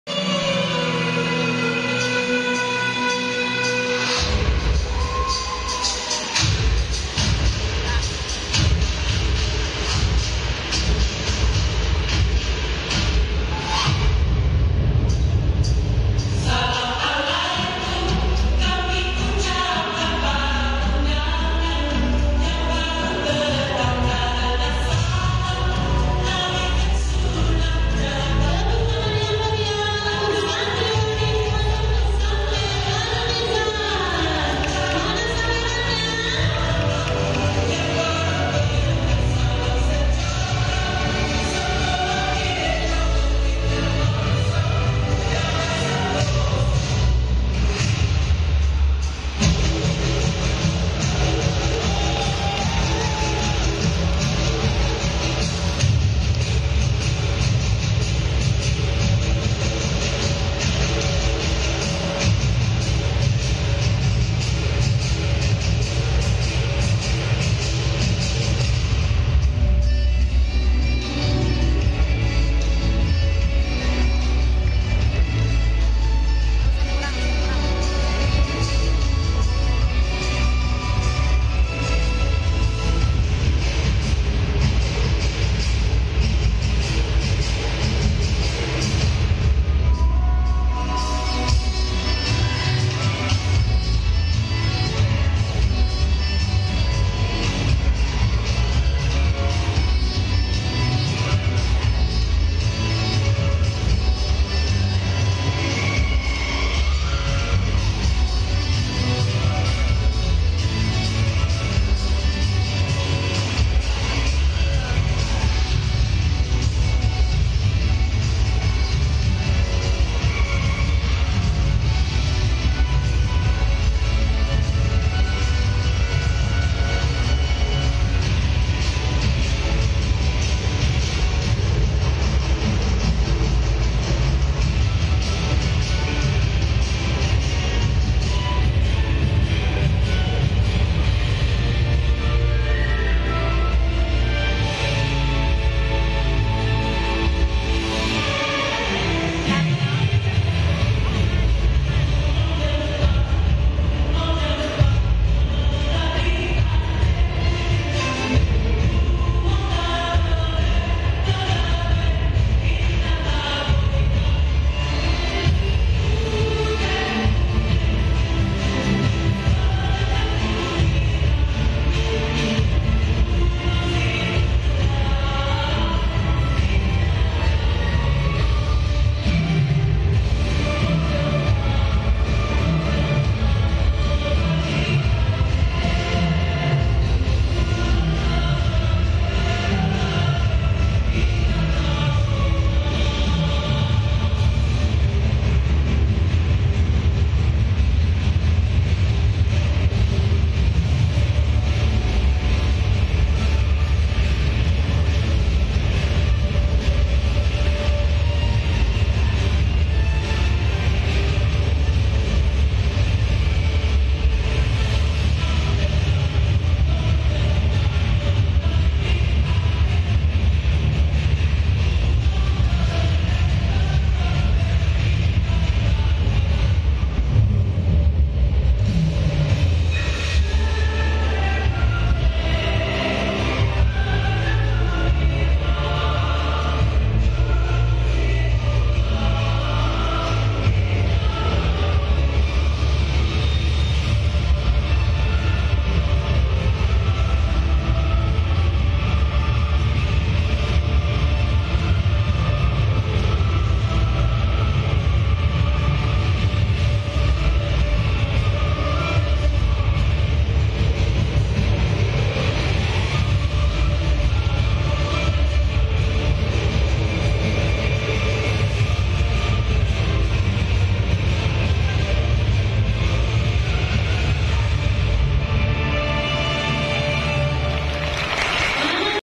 Tari Saman kreasi tari Fatayat Nu Majalengka
Assalamualaikum wrb..

vidio tarian saman oleh fatayat nu majalengka